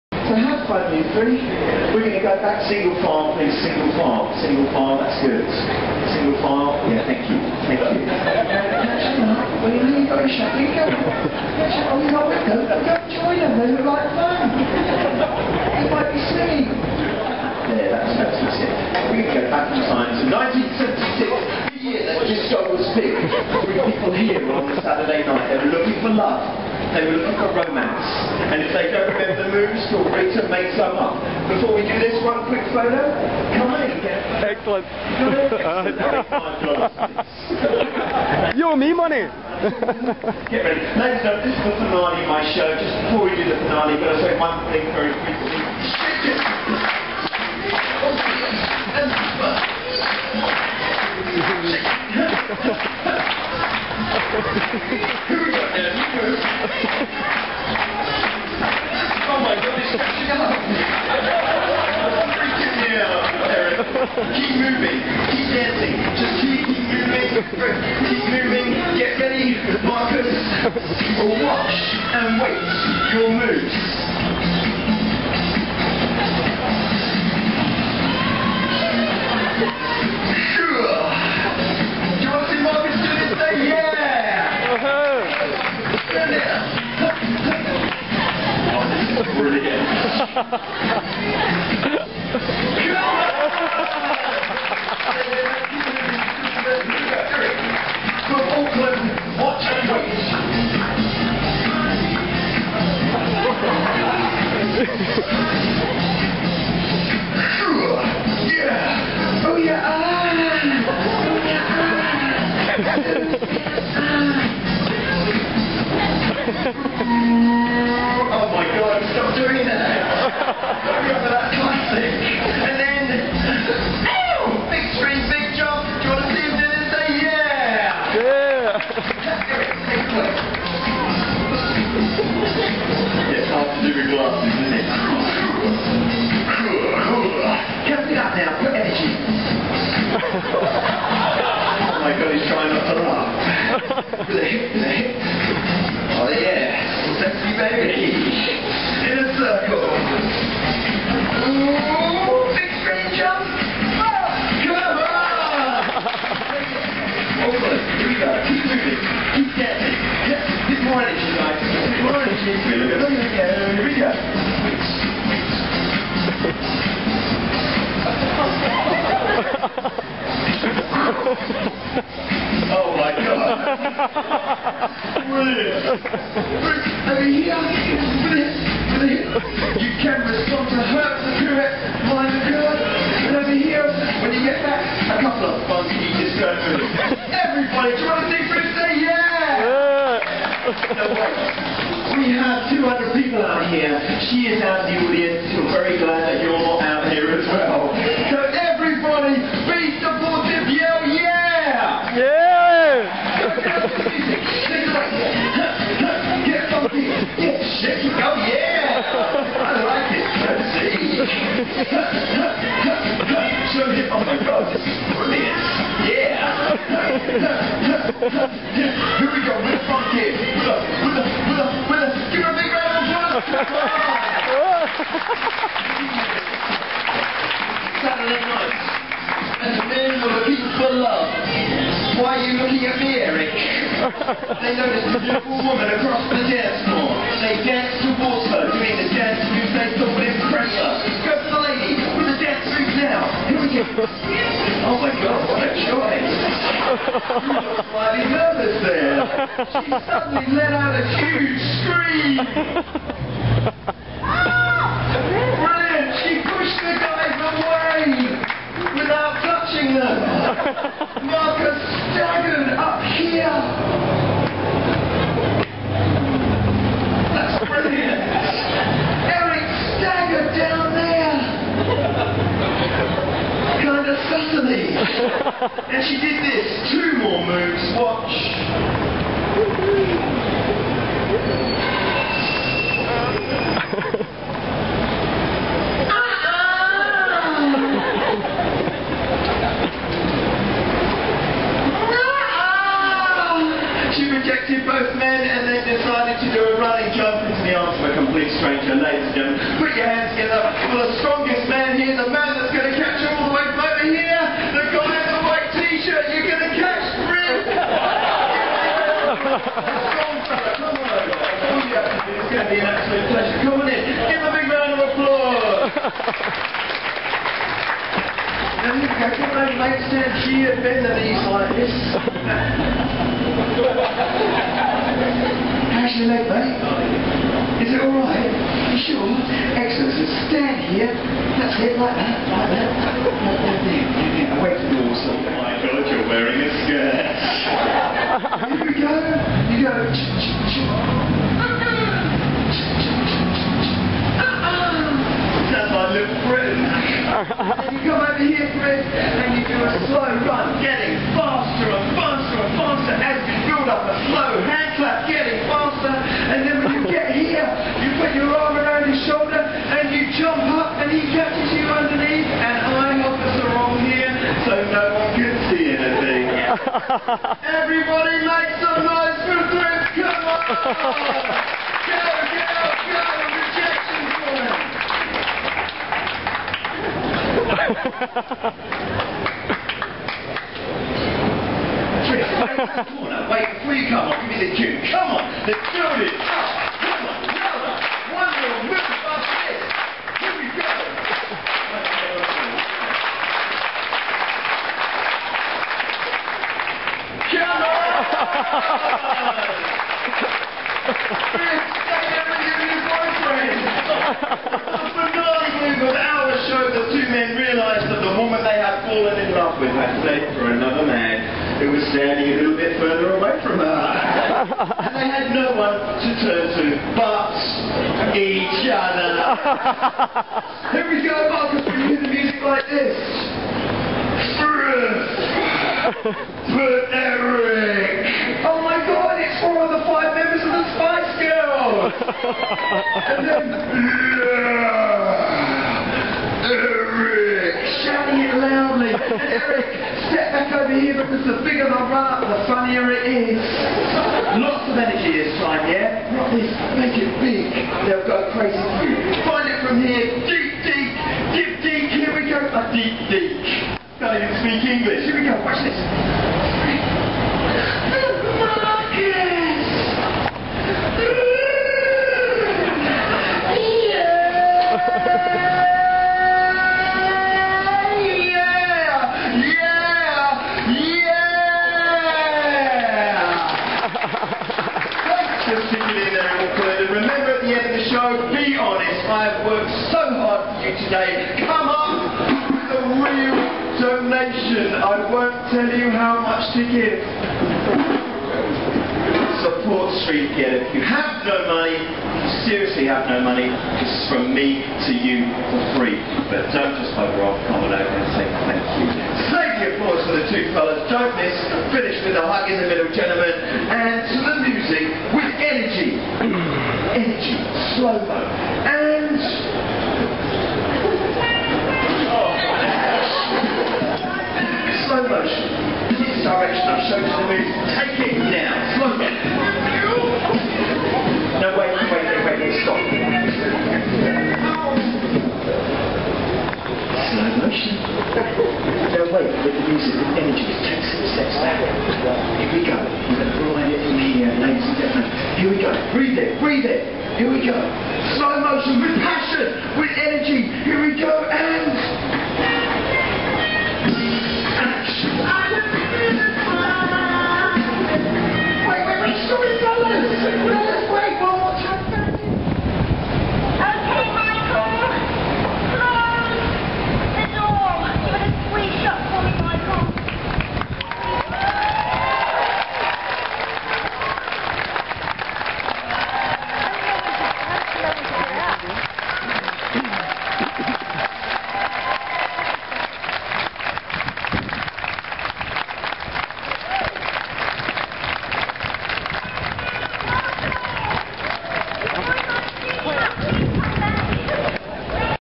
frith at the busker festival
Frith takes part with a Busker. Bad camera work